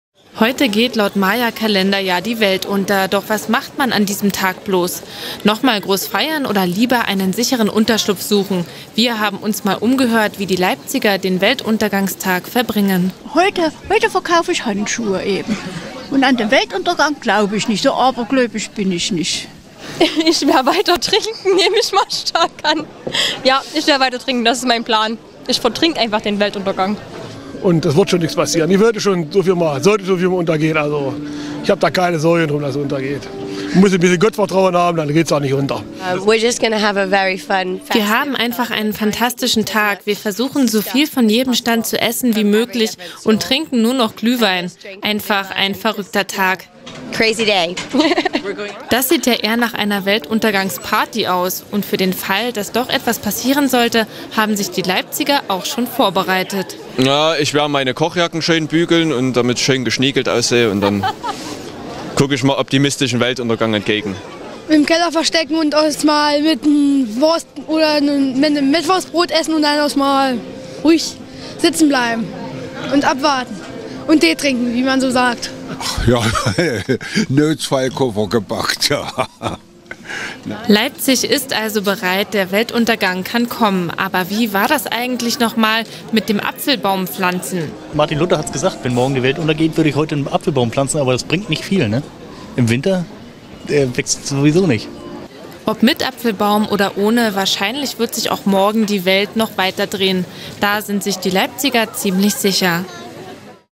Was machen die Leipziger am Weltuntergangstag?
Leipzig, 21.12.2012: Heute geht die Welt unter -- und die Leipziger nehmen es locker. Viele wollen feiern aber so mancher hat auch eine besondere Idee für diesen Tag.